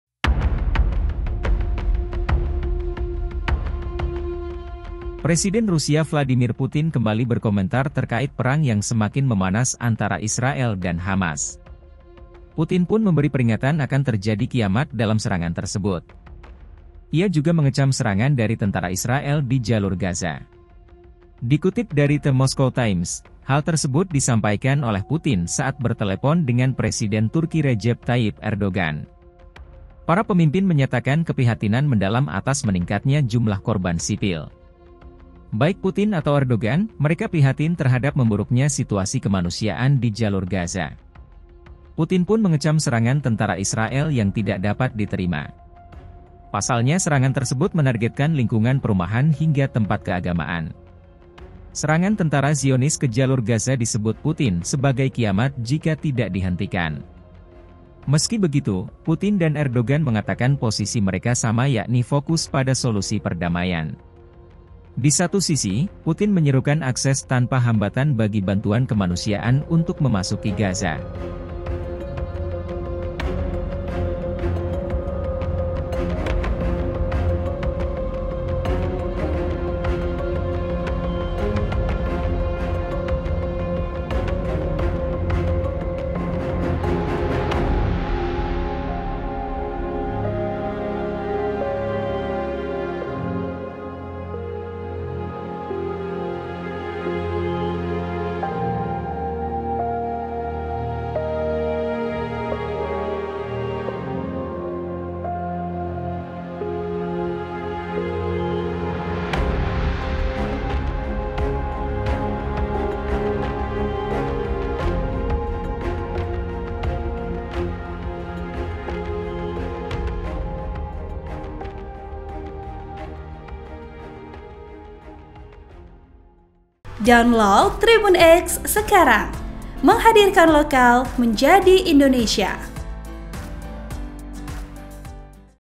PUTIN KECAM Tentara Zionis Israel & Serukan Peringatan KIAMAT di Gaza Jika Serangan Tak Dihentikan!
TRIBUN-MADURA.COM - Presiden Rusia Vladimir Putin kembali berkomentar terkait perang yang semakin memanas antara Israel dan Hamas.

Putin pun memberi peringatan akan terjadi kiamat dalam serangan tersebut.

Ia juga mengecam serangan dari tentara Israel di Jalur Gaza.

Dikutip dari The Moskow Times, hal tersebut disampaikan oleh Putin saat bertelepon dengan Presiden Turki Recep Tayyip Erdogan.

Para pemimpin menyatakan keprihatinan mendalam atas meningkatnya jumlah korban sipil.

Baik Putin atau Erdogan, mereka prihatin terhadap memburuknya situasi kemanusiaan di Jalur Gaza.

Putin pun mengecam serangan tentara Israel yang tidak dapat diterima.

Pasalnya serangan tersebut menargetkan lingkungan perumahan hingga tempat keagamaan.

Serangan tentara Zionis ke jalur Gaza disebut Putin sebagai kiamat jika tidak dihentikan.

Meski begitu, Putin dan Erdogan mengatakan posisi mereka sama yakni fokus pada solusi perdamaian.

Di satu sisi, Putin menyerukan akses tanpa hambatan bagi bantuan kemanusiaan untuk memasuki Gaza.

VP: Yogi